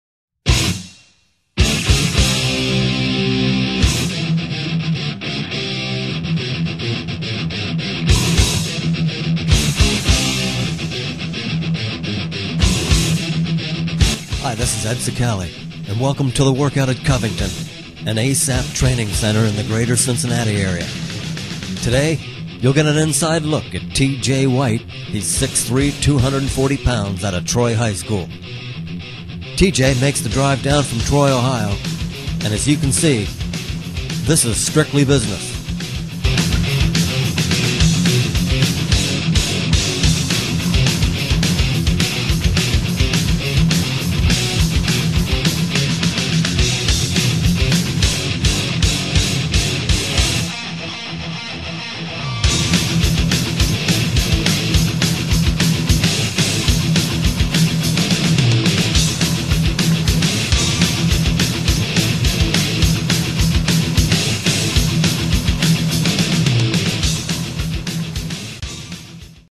ASAP Blogs: Speed and Agility Training Video; T.J. White
This is T.J. White of Tryo High School performing a training session at The Workout@Covington located in the greater Cincinnati area. actually in Covington, Kentucky.